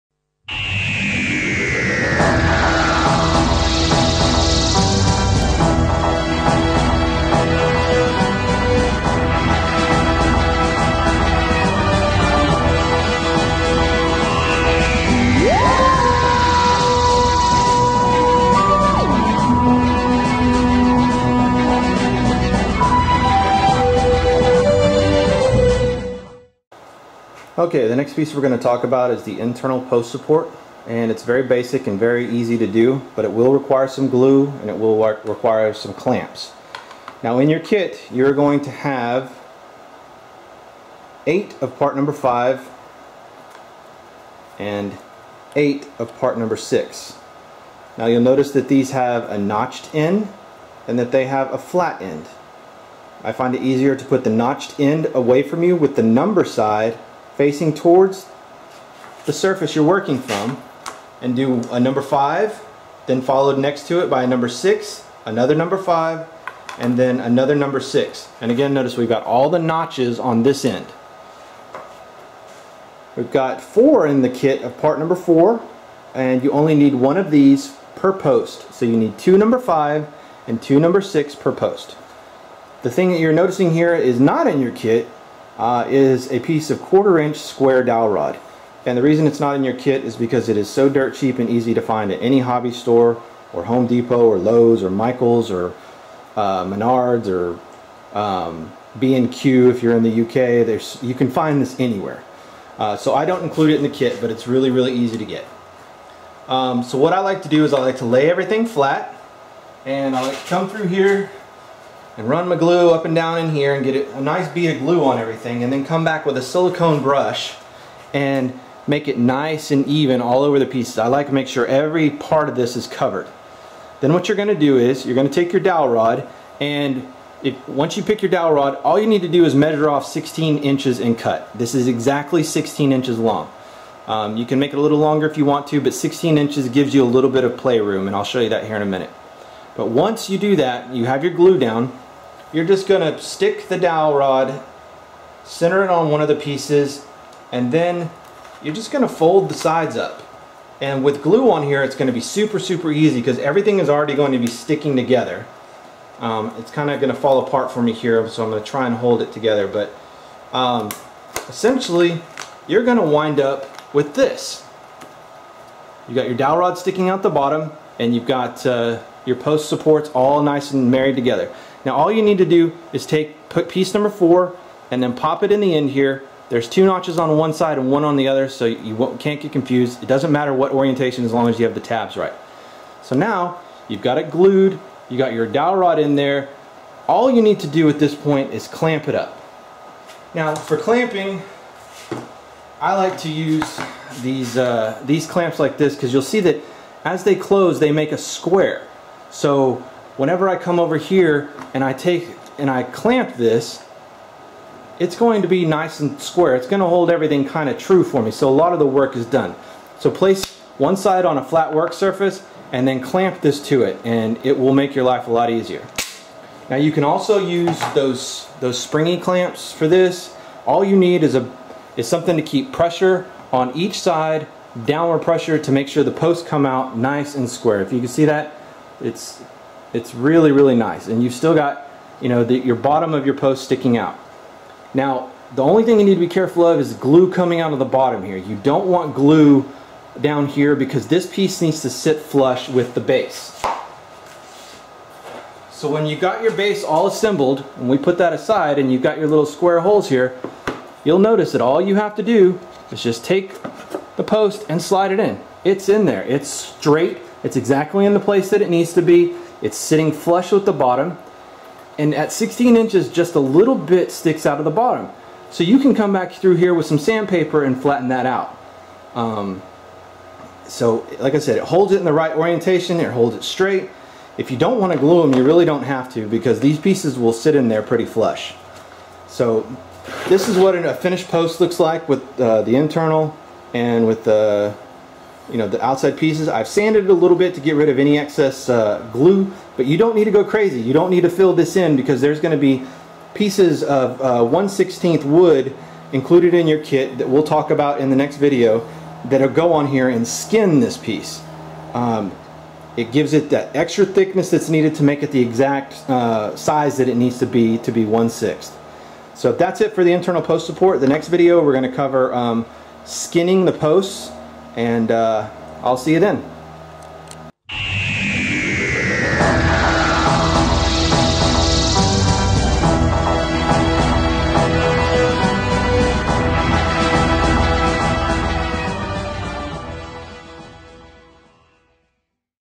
Building the 1/6 Scale "Big Blue" TARDIS Part 2
Here is Part II of the series showing how to assemble this kit.  This is the 1/6 Scale "Big Blue" TARDIS kit.  Part II covers assembling the internal post supports.  It is all laser cut wood, with 3D printed accent parts.